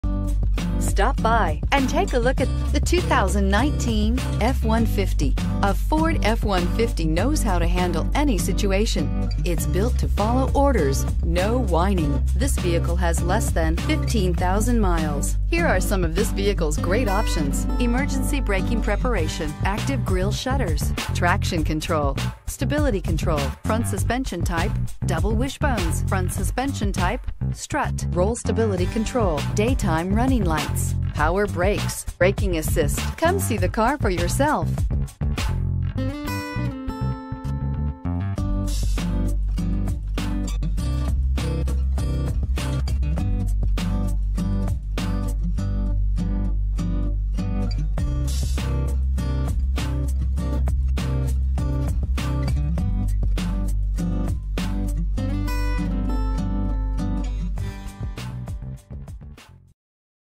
2019 Ford F-150 New Bern NC T11508A
2019 Ford F-150 XL

Cella Ford
3210 Doctor M.L.K. Jr Blvd

Get ready to go for a ride in this 2019 Ford F-150 XL, which comes equipped with a braking assist, hill start assist, stability control, traction control, anti-lock brakes, dual airbags, side air bag system, digital display, and airbag deactivation.  It has a 6 Cylinder engine.  We're offering a great deal on this one at $32,097.  Drive away with an impeccable 5-star crash test rating and prepare yourself for any situation.  For a good-looking vehicle from the inside out, this car features a timeless magnetic metallic exterior along with a black / medium earth gray interior.  Call or visit to talk with one of our vehicle specialists today!